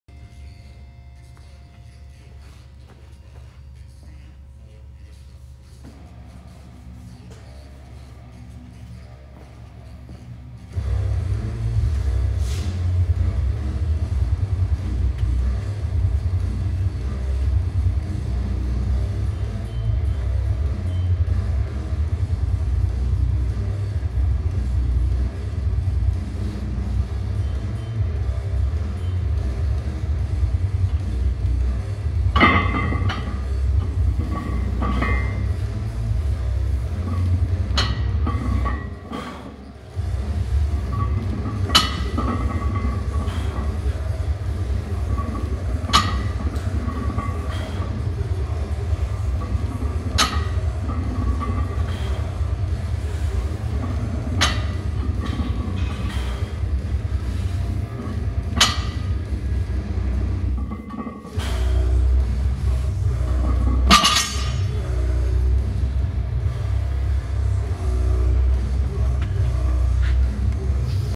Snatch grip deadlifts day 4
Snatch grip deadlifts on three inch blocks at 225